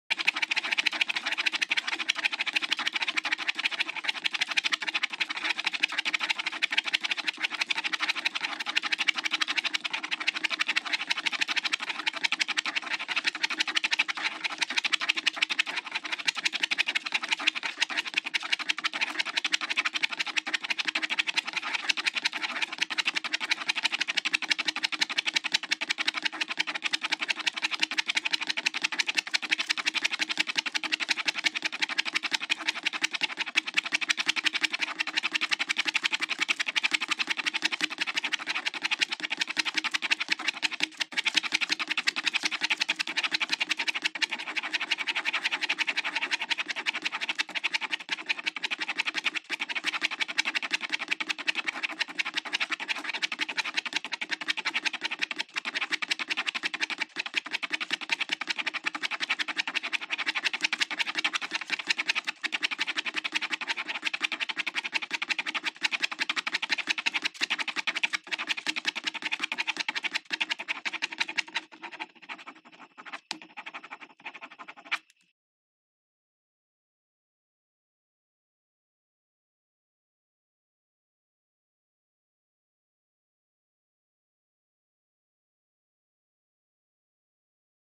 Drawing an underwater scene- scuba diver, clown fish, coral
Colored Pencil Drawing of my friend scuba diving in the Great Barrier Reef.